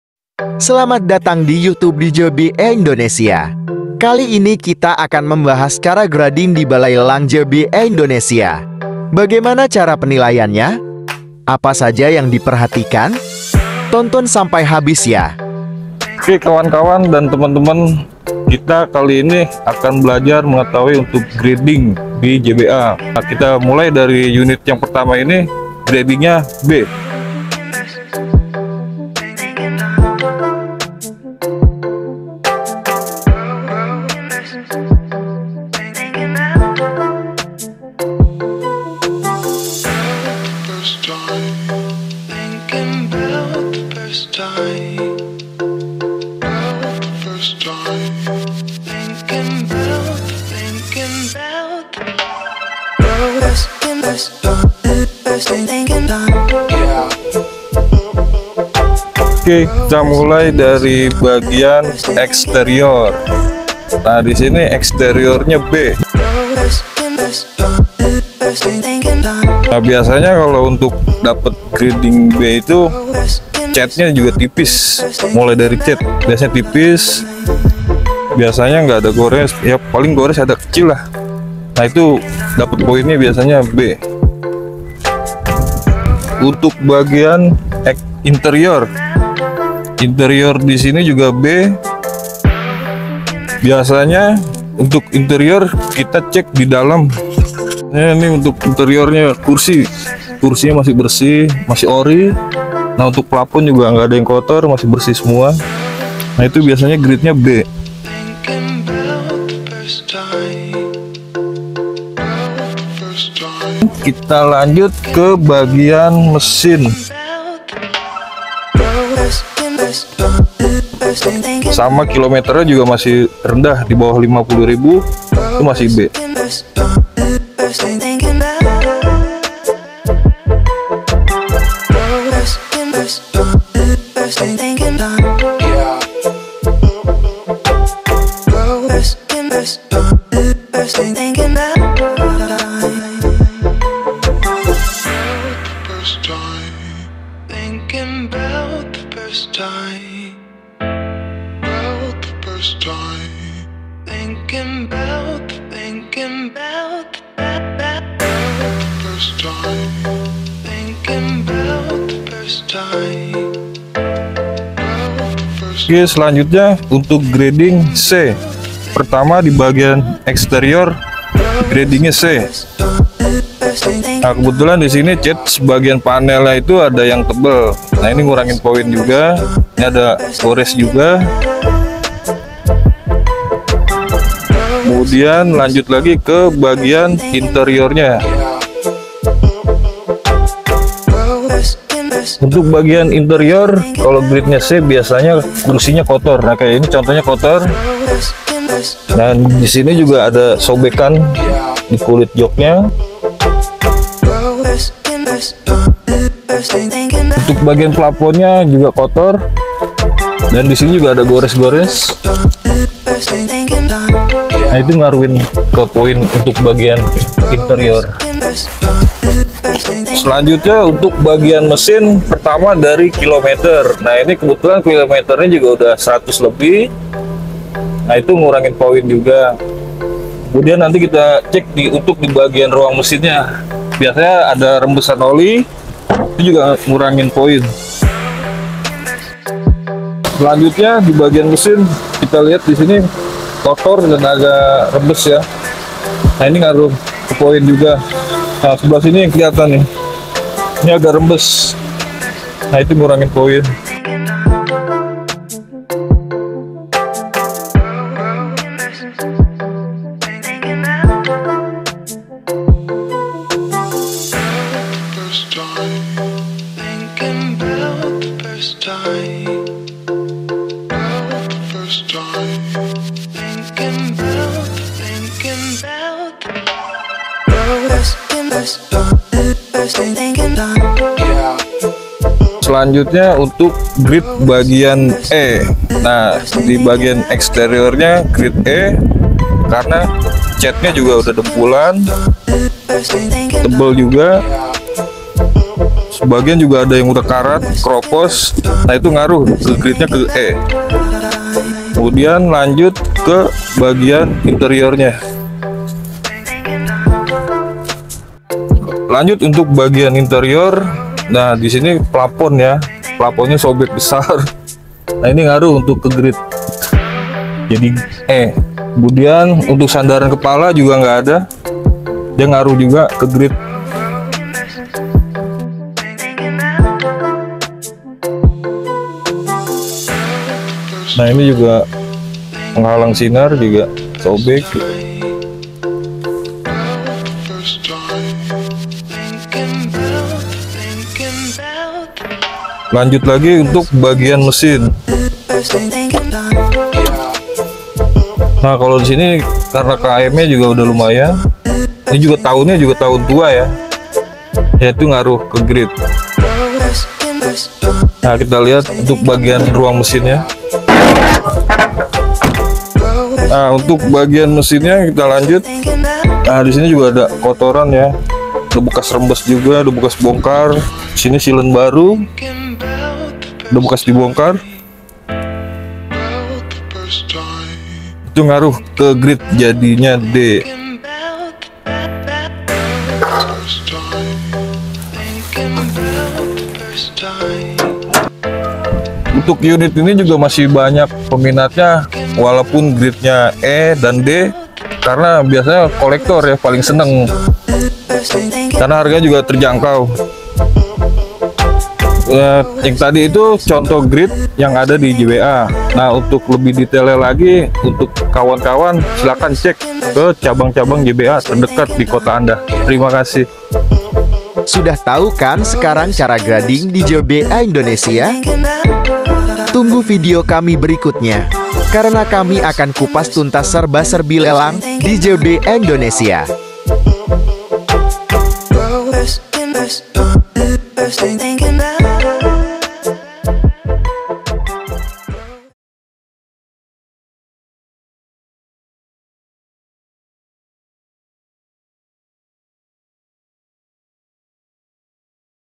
Ini Dia Cara Grading Unit di JBA Indonesia!
Gimana sih cara grading unit di JBA Indonesia bisa dapat grade B hingga F? Tonton sampai habis, karena FI JBA bakal kasih penjelasan buat kamu semua, biar bisa mengerti nih!